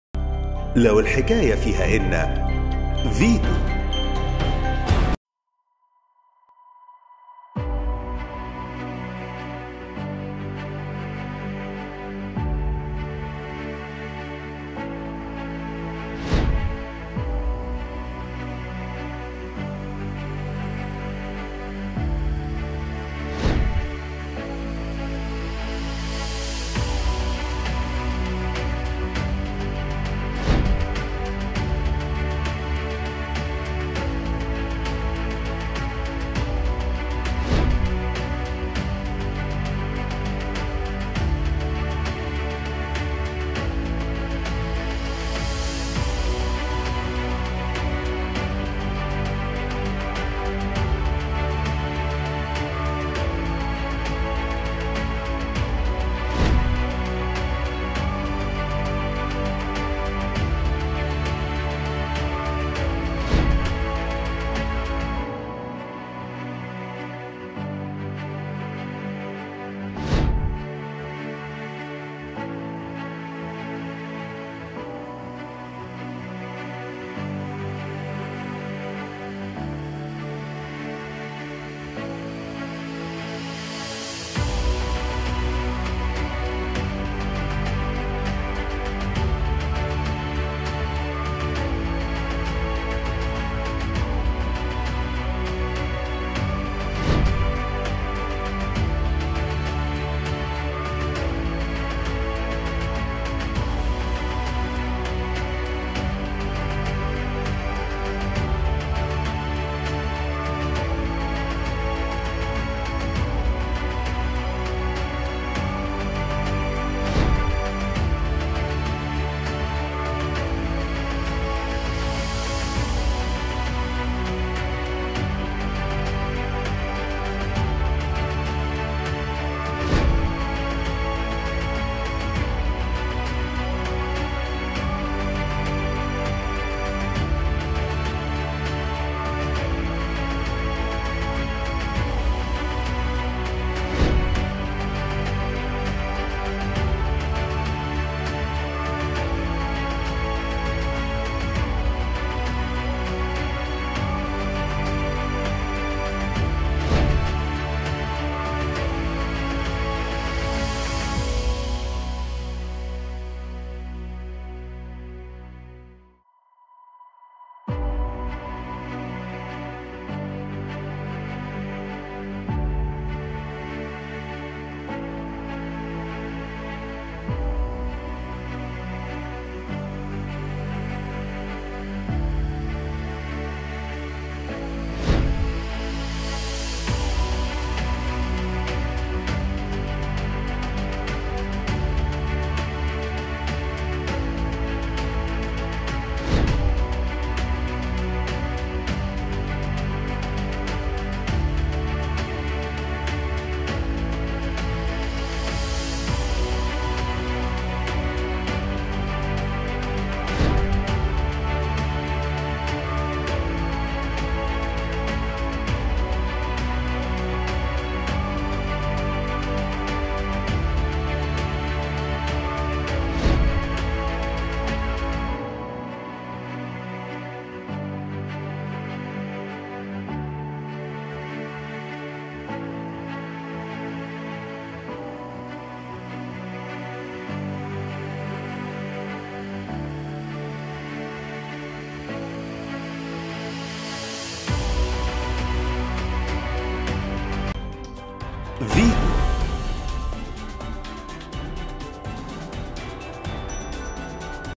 الملك فاروق بين مواجهة الإنجليز وفضائح الأم
فاروق.. في مواجهة الإنجليز وغدر الأم وظلم التاريخ